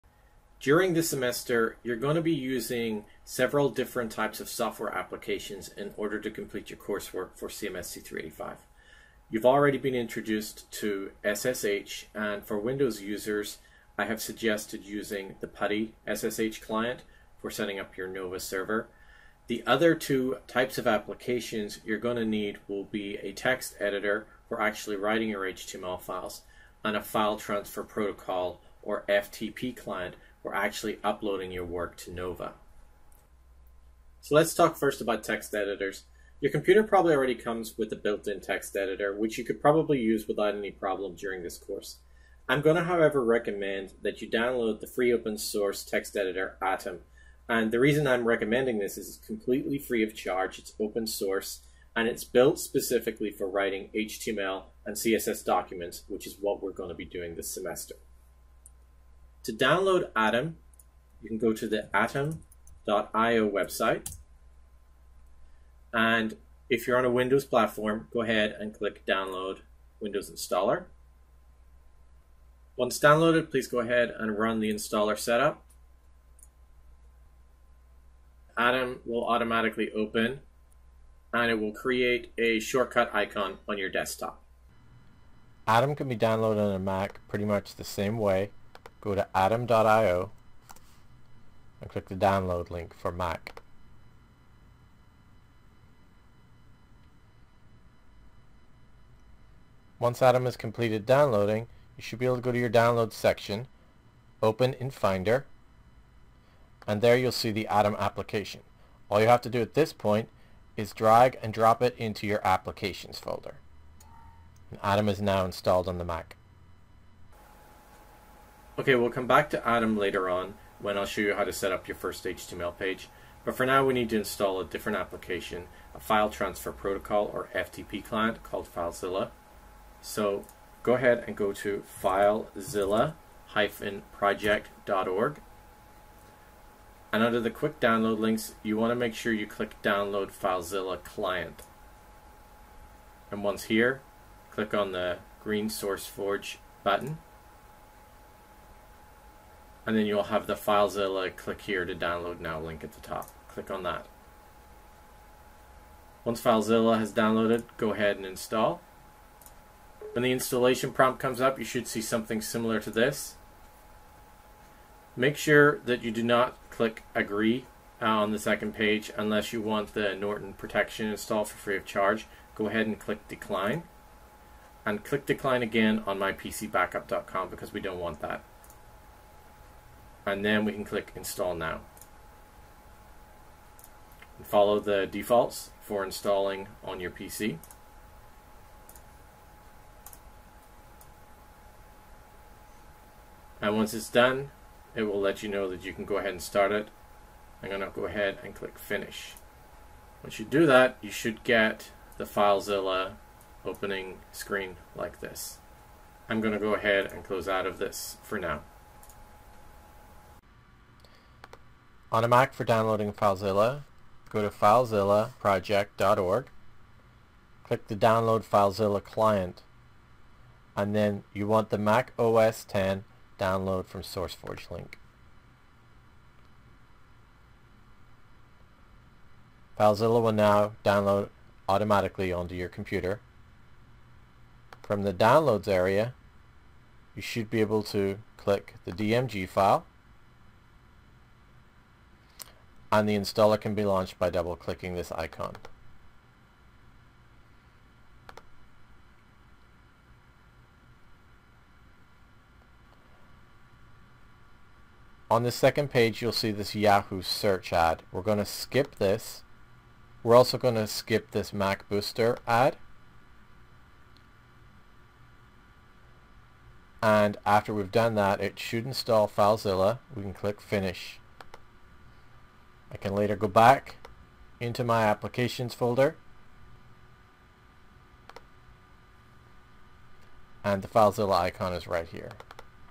UMUC Web Server Setup Part 2 - Software Setup
Walkthrough for setting up UMUC web server for CMST 385, 386, 388, and 488.